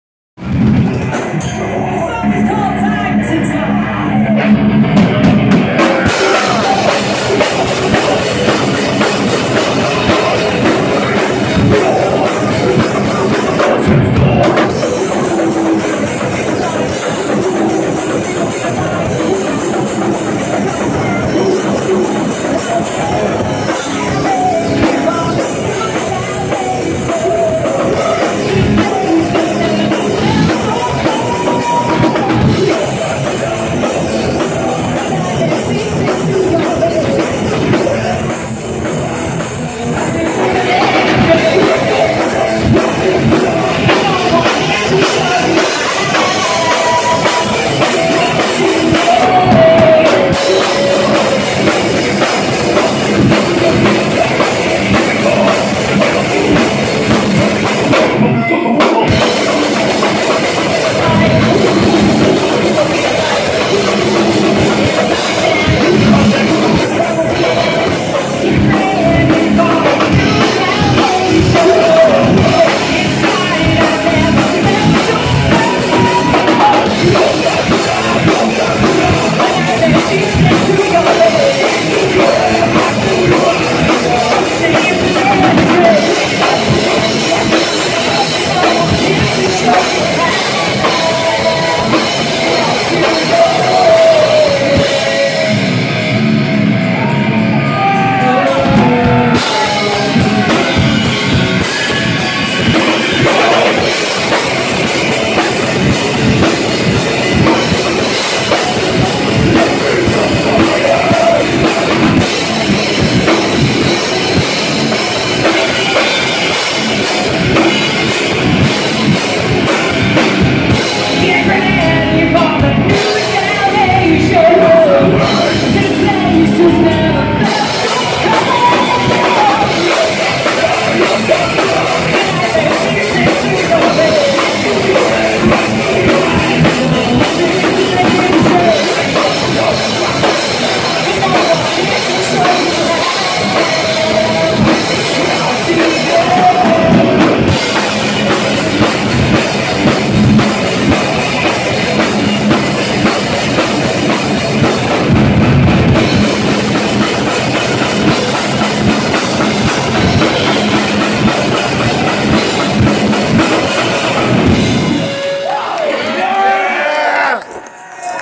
Devils Playground at Sophiefest ARCHES VENUE COVENTRY 15/07/16
Devils Playground are a Punk Band

Link's are below for the Band and people who have helped set up sophiefest for the Sophie Lancaster Foundation

Recorded on my phone